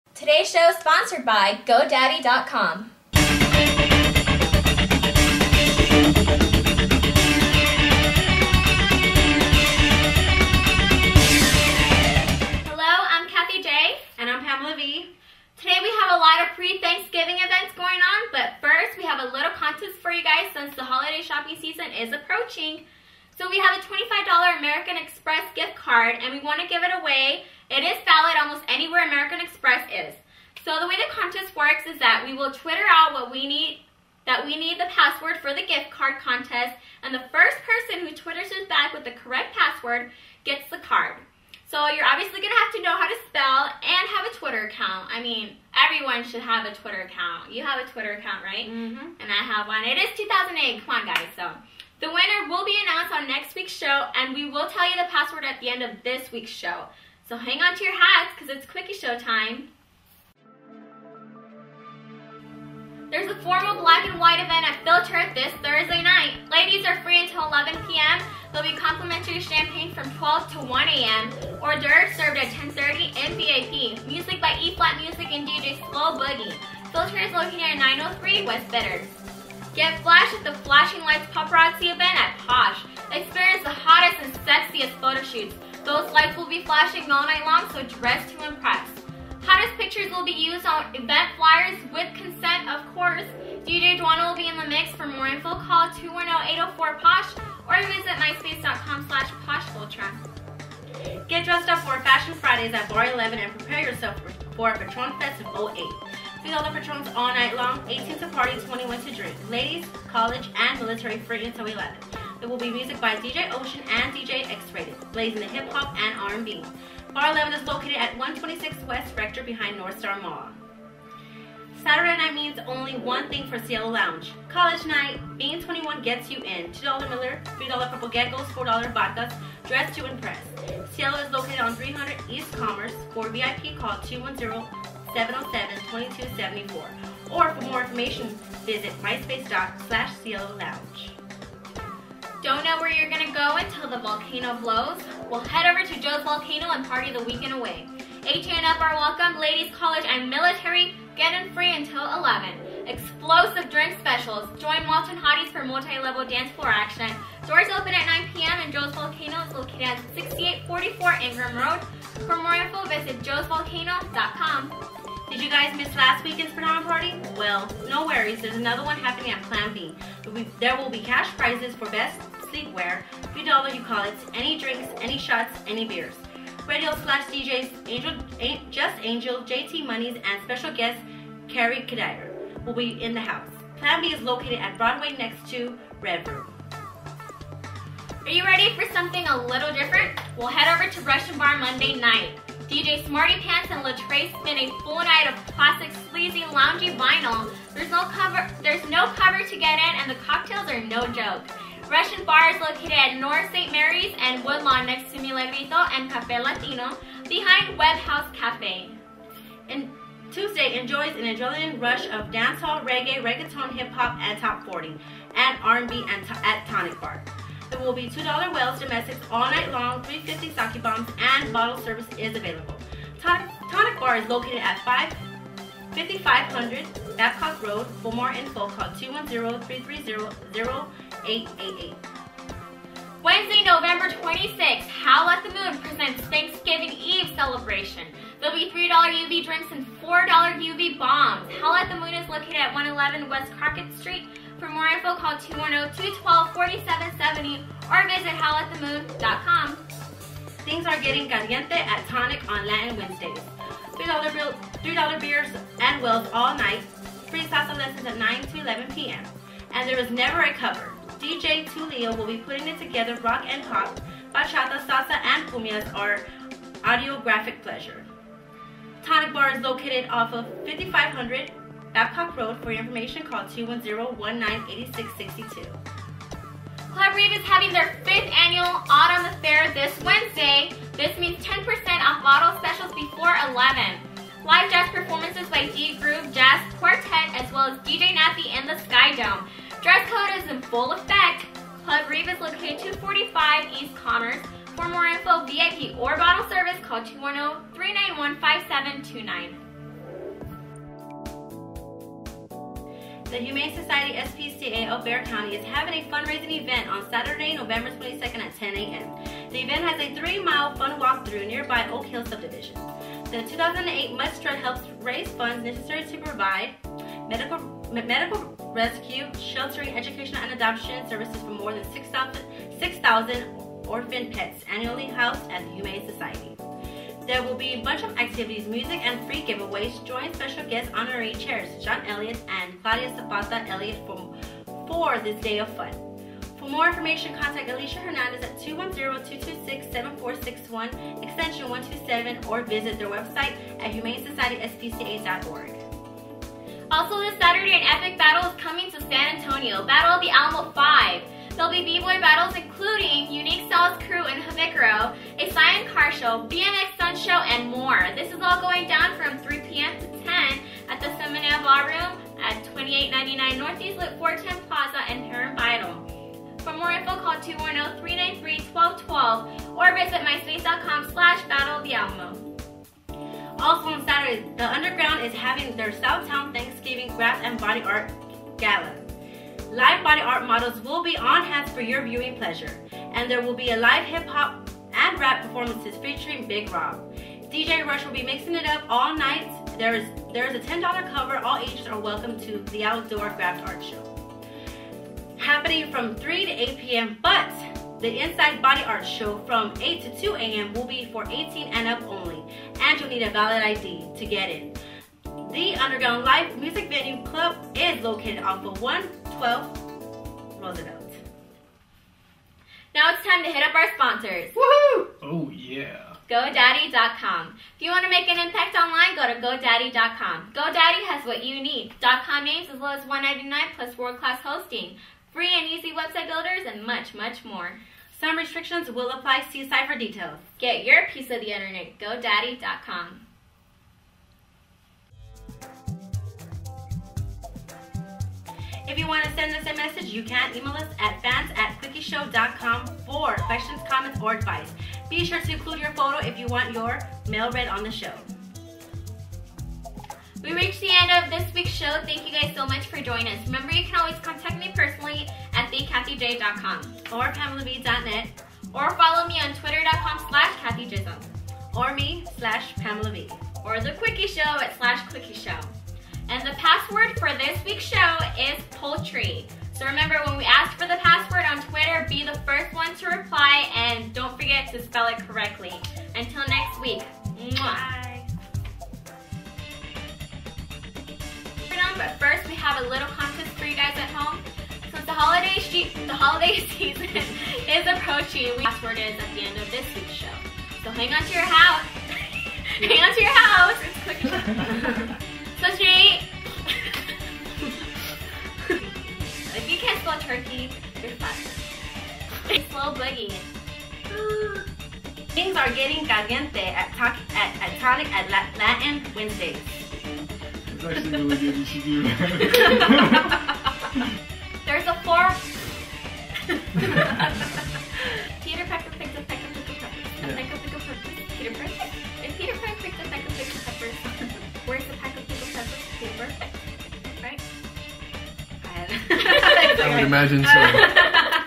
Quicky Show - Episode 12 - 11-19-2008
We have a bunch of night life activities and other events this Saturday as well as a $25 Gift card give away! So hold onto your house, the Quicky Show is here!